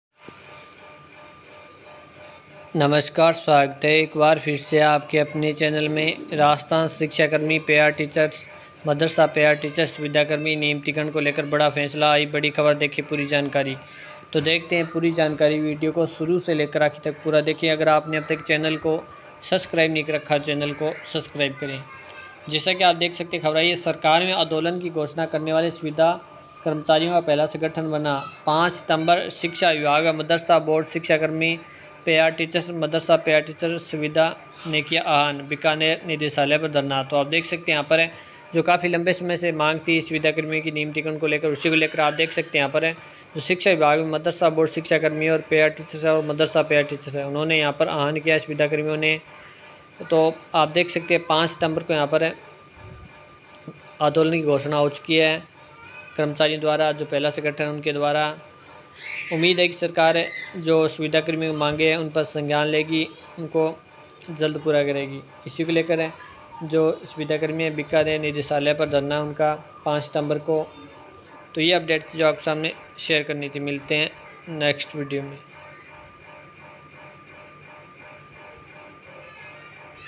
राजस्थान शिक्षाकर्मी,पैरा मदरसा पैराटीचर्स संविदा कर्मी नियमितीकरण बड़ी खबर,Rajasthan samvida karmi
राजस्थान शिक्षा कर्मी पैरा टीचर्स,मदरसा पैरा टीचर्स संविदा कर्मी नियमितीकरण बड़ा फैसला आई बड़ी खबर || Rajasthan samvida karmi news